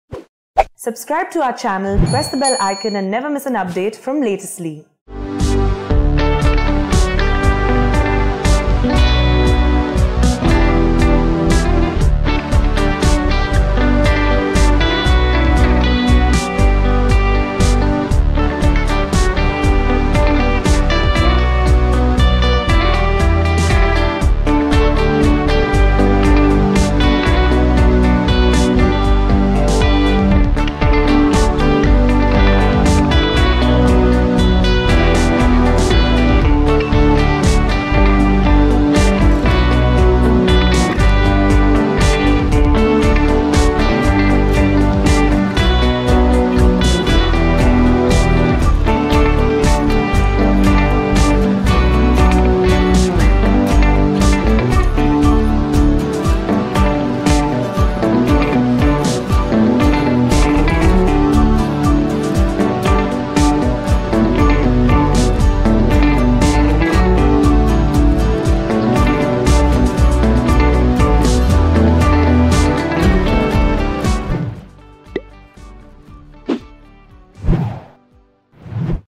Irfan Pathan Birthday Special: Lesser-Known Things About Former Swing Specialist of Indian Cricket
Irfan Pathan celebrates his 35th birthday on October 27, 2019 (Sunday). The former Indian bowler from Gujarat was the star bowler in ICC T20I World Cup 2007, helping Team India to lift the title. A fast-medium swing and seam bowler, Pathan made his international debut at the age of 19.  Currently, Pathan is an active commentator.